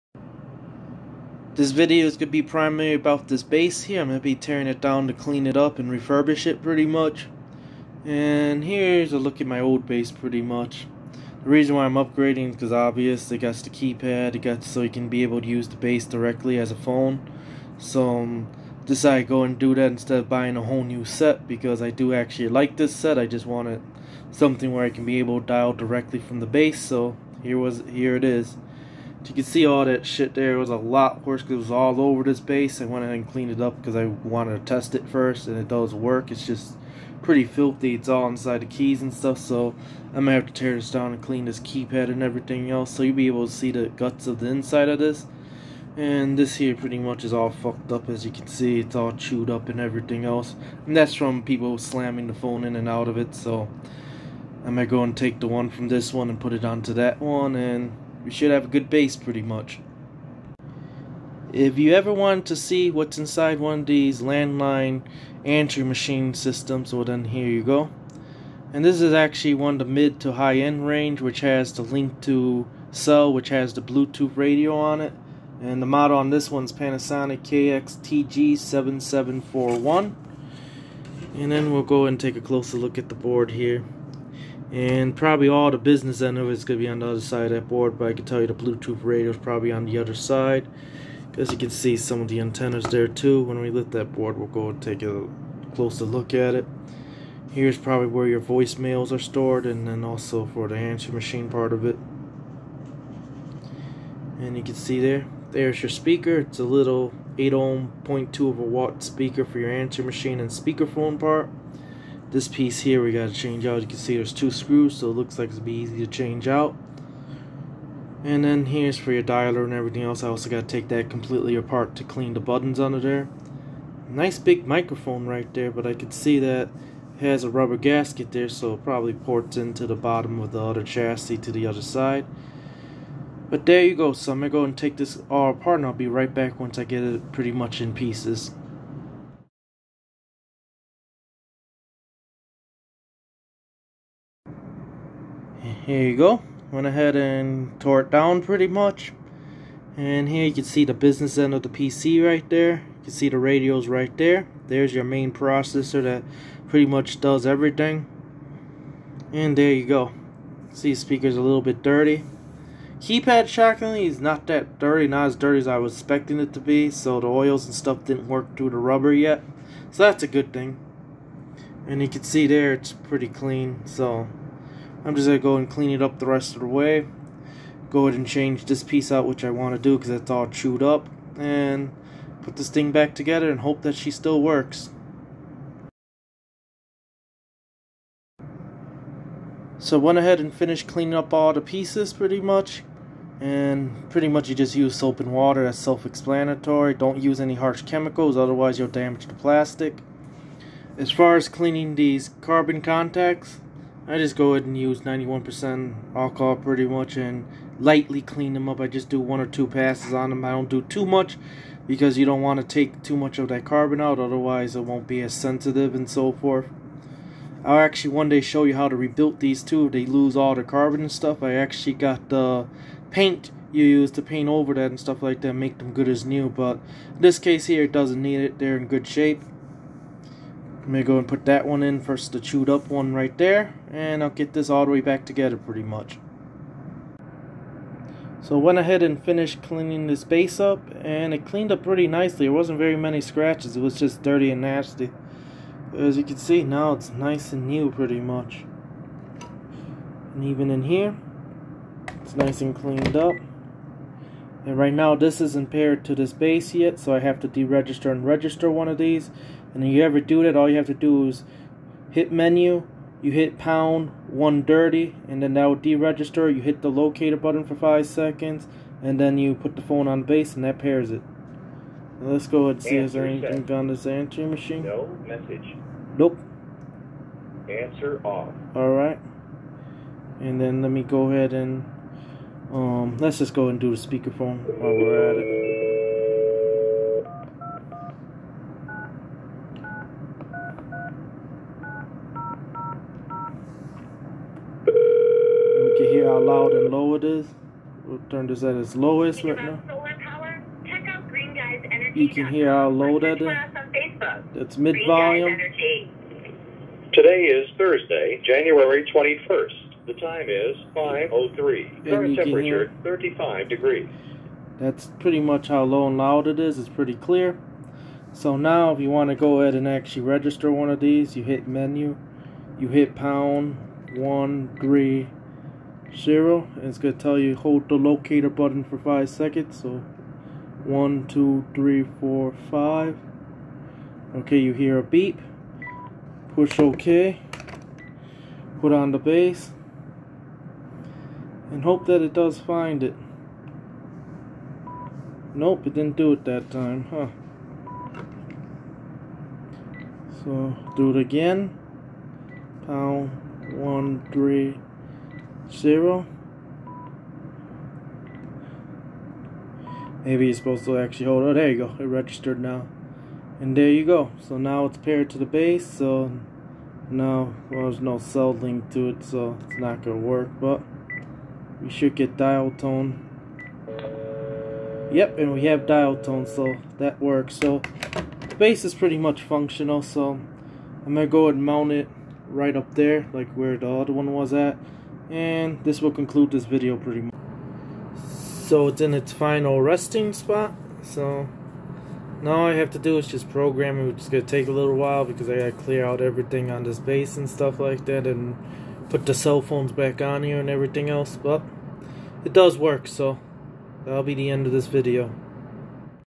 Panasonic KX-TG7741 Cordless Phone Base Teardown & Restoration.
Cleaning up a Cordless Phone Base and figured it make for a good teardown video.